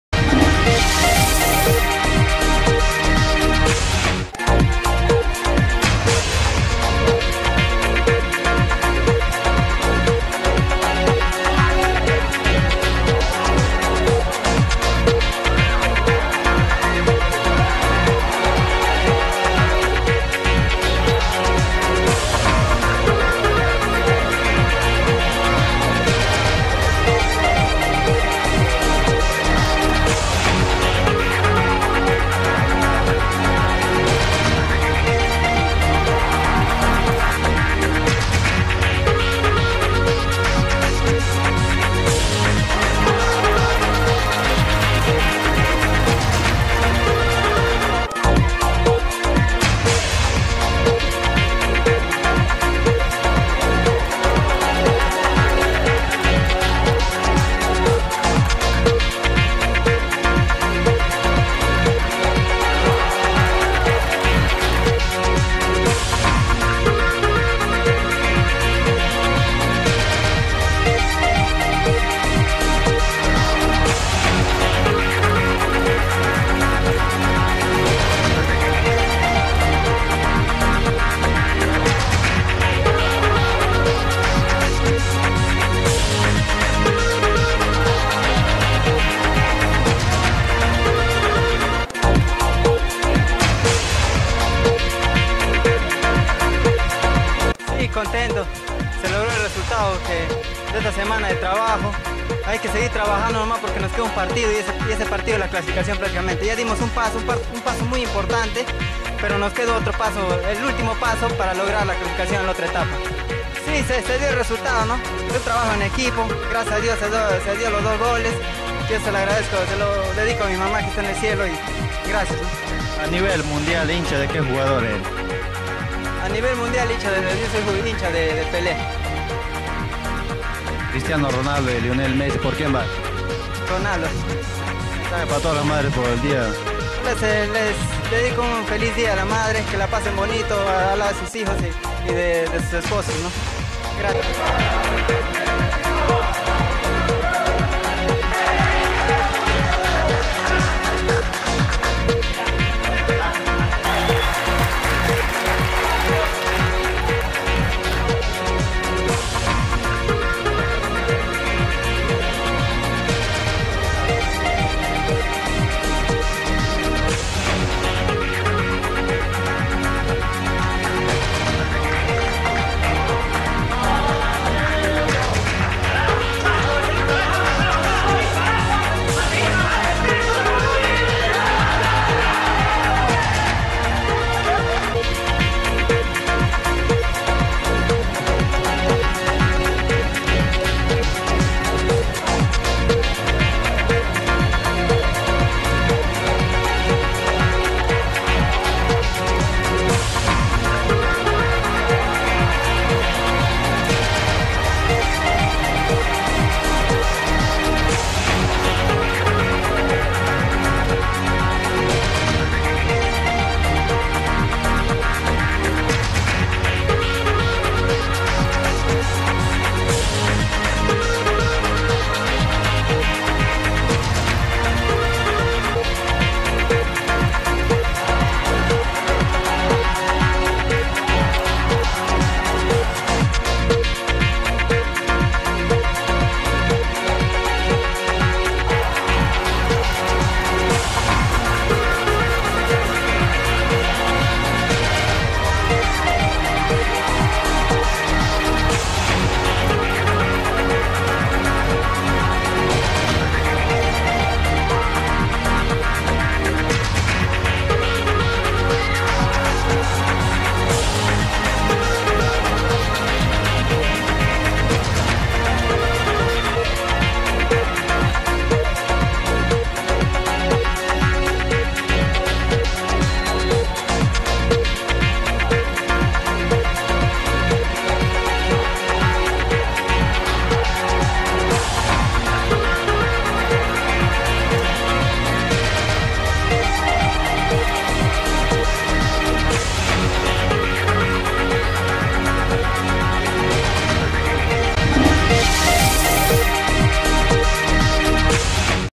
Cuarta Fecha - Etapa Distrital de Callería - Resumen y Entrevista - Copa Perú 2014
Deportivo Municipal vs Defensor Pucallpa
Santa Rosa vs UNU
Deportivo Bancos vs Hospital
Universidad Nacional de Ucayali vs Santa Rosa
Deportivo Municipal vs UNU